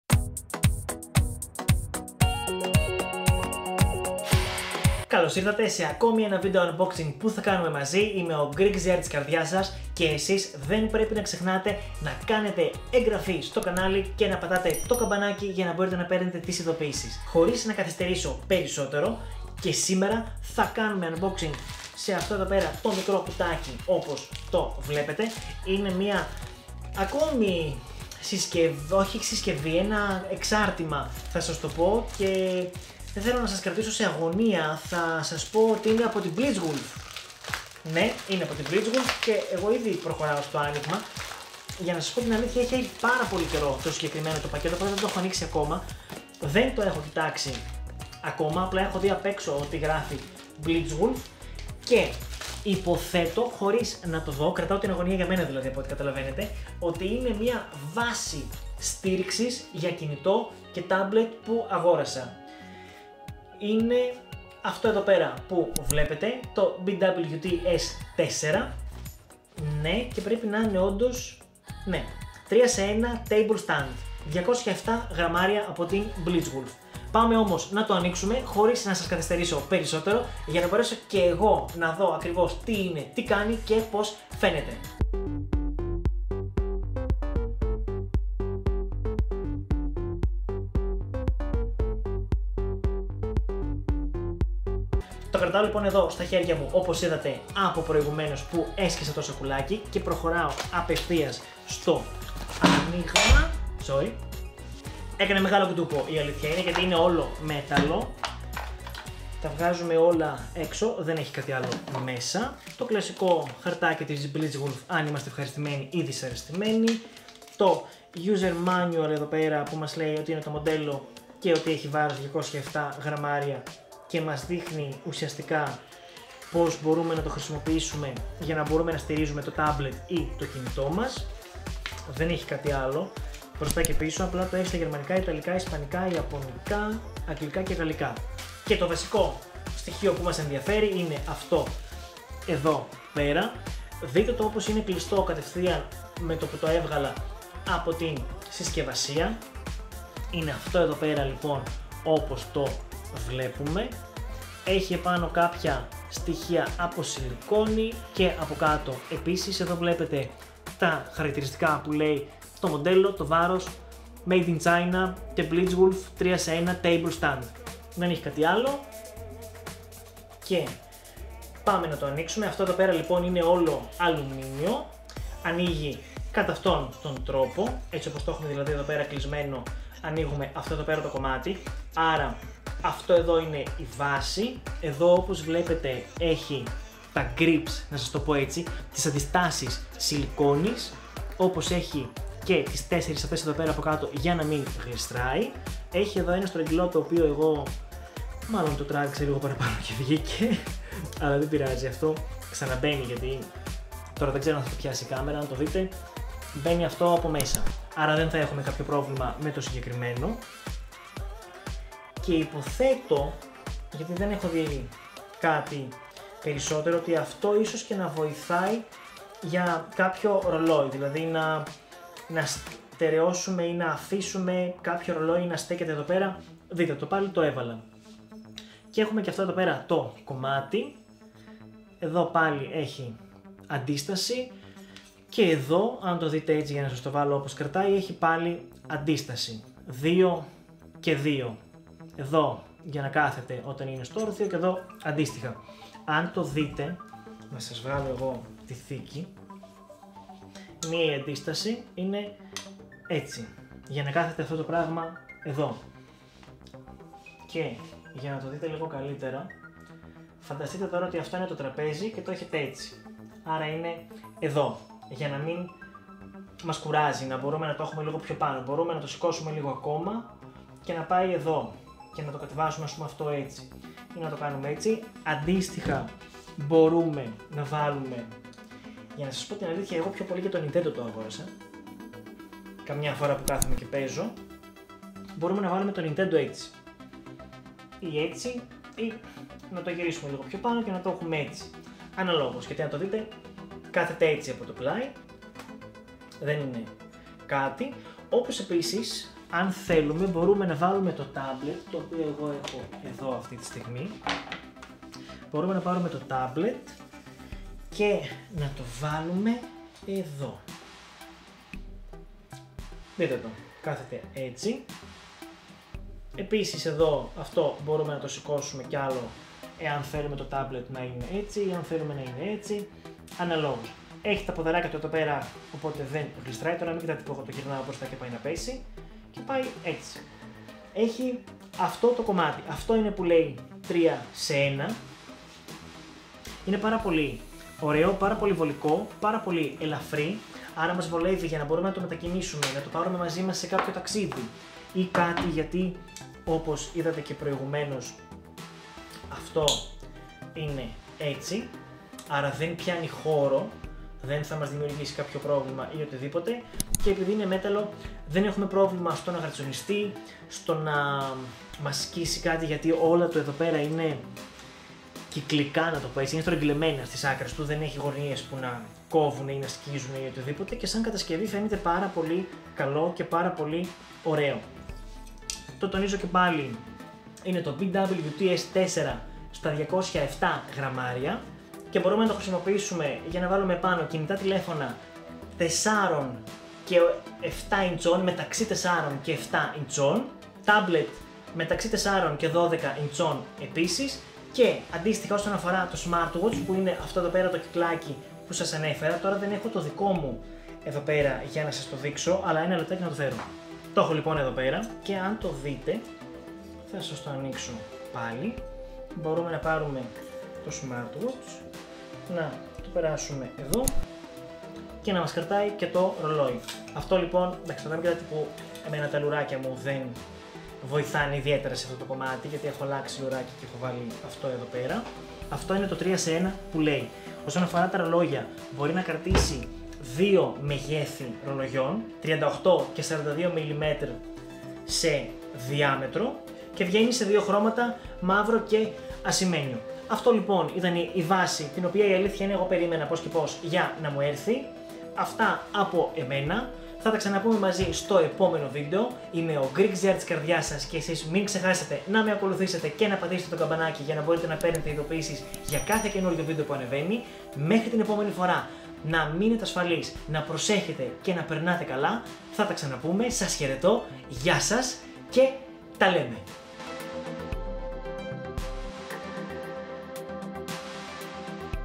BlitzWolf BW-TS4 3 in 1 Table Stand unboxing (eng subs)
Ελ.: Ανοίγω για να δούμε την βάση στήριξης για ταμπλετ, κινητό και ρολόι χειρός της BlitzWolf. Είναι το μοντέλο BW-TS4. Ένα αντικείμενο το οποίο είναι κατασκευασμένο από αλουμίνιο, σε μαύρο χρώμα στην δική μου περίπτωση, βάρους 207 γραμμαρίων. Αποτελείται από τρία μέρη (βάση, στήριξη κινητού - Nintendo Switch και, στήριξη ταμπλετ).
Δείτε όλες τις λεπτομέρειες στο βίντεο.

En.: I am unboxing the 3 in 1 table stand from Blitzwolf, BW-TS4. It's a stand made of aluminum, in black color in my case, weighing 207 grammars. It's made of three parts (the base, the mobile phone - Nintendo Switch stand and, the tablet stand).
See all details in the video.